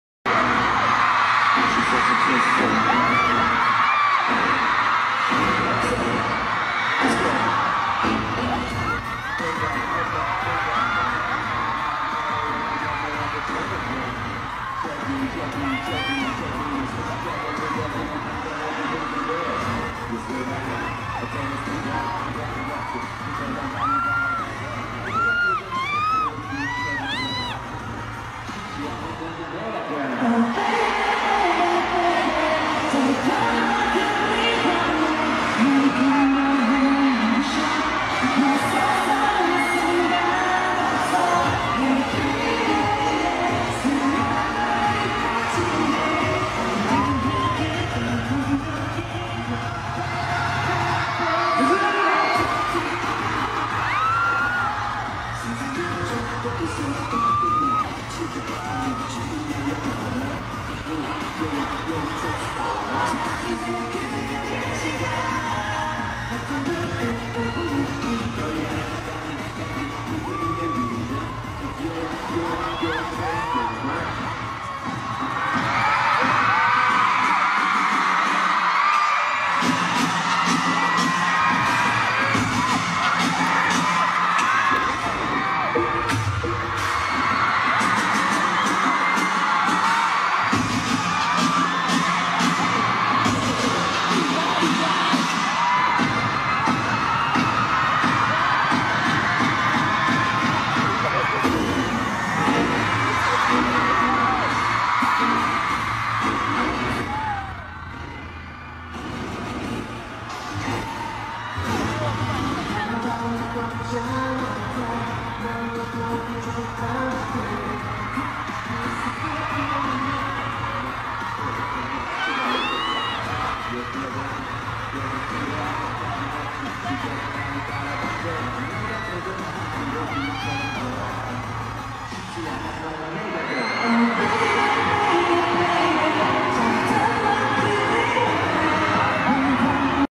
160212 EXO Chen ripped his pants during MAMA 2016 performance
Chen showed professionalism.